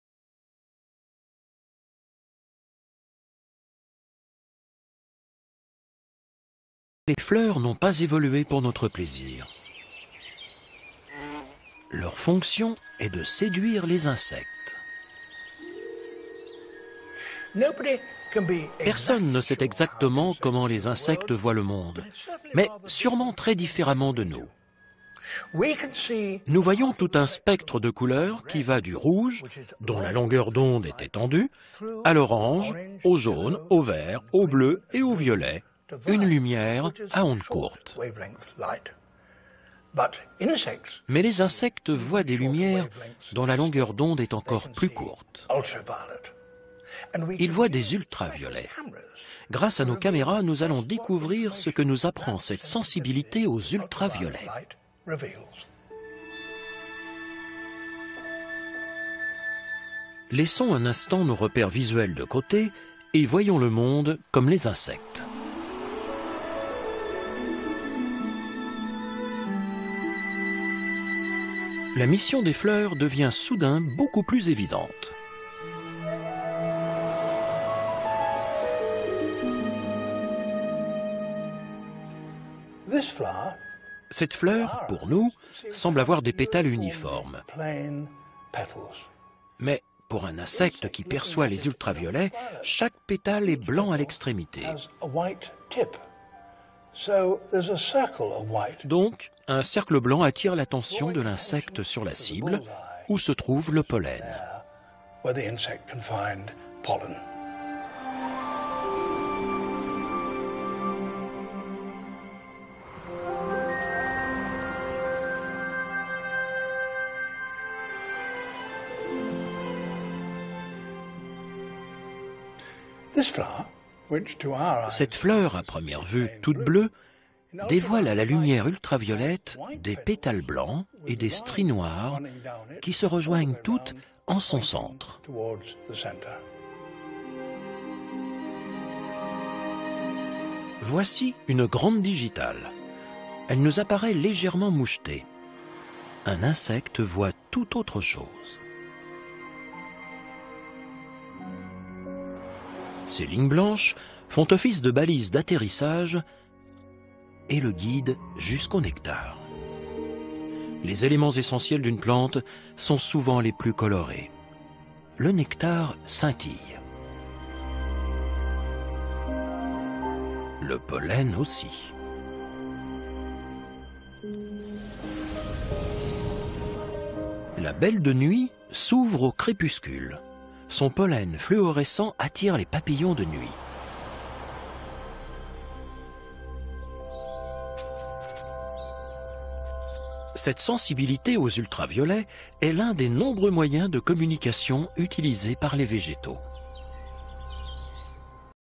Vraies couleur des plantes 3min18
La vrai couleur des plantes tel que les plantes veulent être vue en particulier par les insectes pollinisateurs.